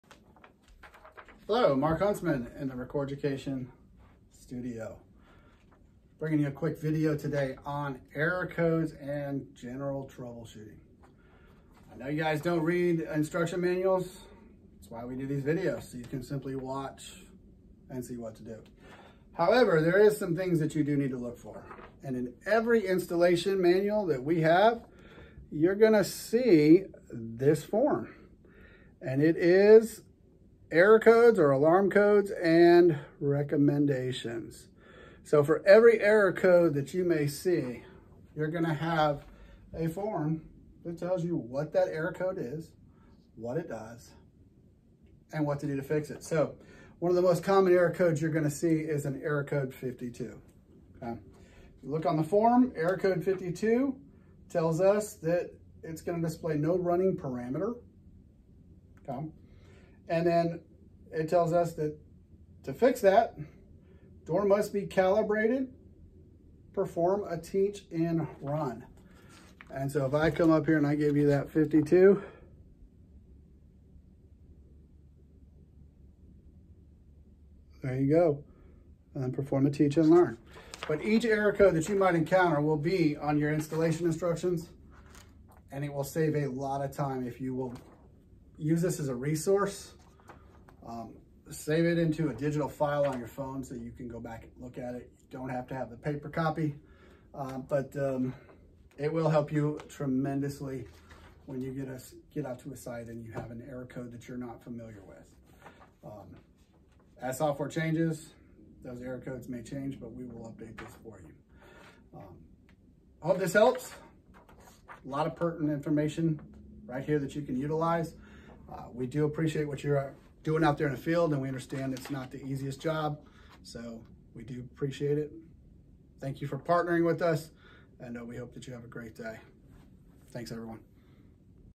Error Codes and Descriptions
Where to find a description of all error codes and potential fixes.